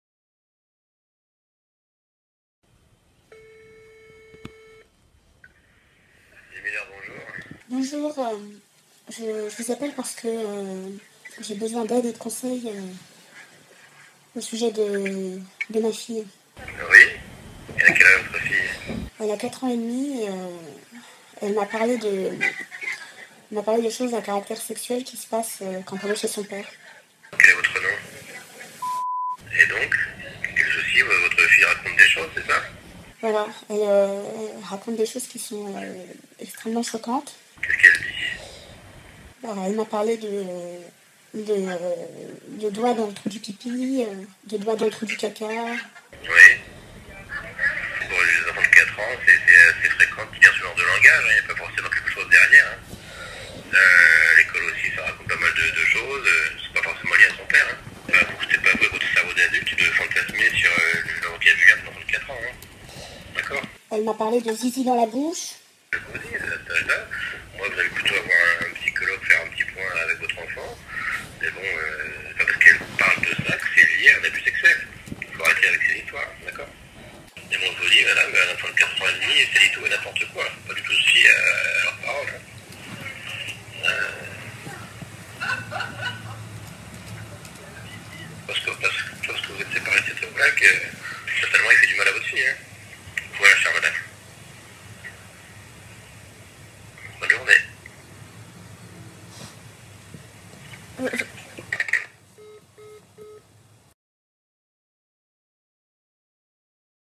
hallucinant 1/5
je ne suis même pas étonné . excusez pour les flics qui ont encore un soupçon de psychologie et de compassion, une écoute attentive,car se que j'entends ici n'est ni acceptable ni tolérable. Un enfant de 4 ans est victime d'un prédateur sexuel déviant et la réponse, à l'appel de cette maman est à vomir. faut pas s'étonner que le monde regarde la France, que l'on nomme pédoland , puisqu' ici, les pédos sont protégés, enfin protégé n'est même pas un terme approprié puisqu'ils sont , en liberté, non soignés de leur deviance,leur perversion.et les victimes n'ont écoutées. c'est un véritable scandale et nous, le peuple, devons dénoncer cela, ce n'est pas acceptable. Pauvres enfants.
Protégez vos enfants et dénoncez toute forme d'abus. cet appel en est un,puisque la réponse de se flic est complètement inconsciente.

Que la paix soit sur vous tous frères et sœurs d'humanité 🌍🙏🏿🧡🍊

la pédophilie, pédocriminalité, pornographie juvénile et à mon sens,( cela ne tient qu'à moi) la pornographie tout cours doit être absolument aboli ,interdit, car c'est trop malheureusement beaucoup en sont adictes et cela peut provoquer des troubles psychologiques graves,surtout si les consommateurs sont jeunes. Donc il faut absolument que tout cela s'arrête et Ensemble nous allons y arriver.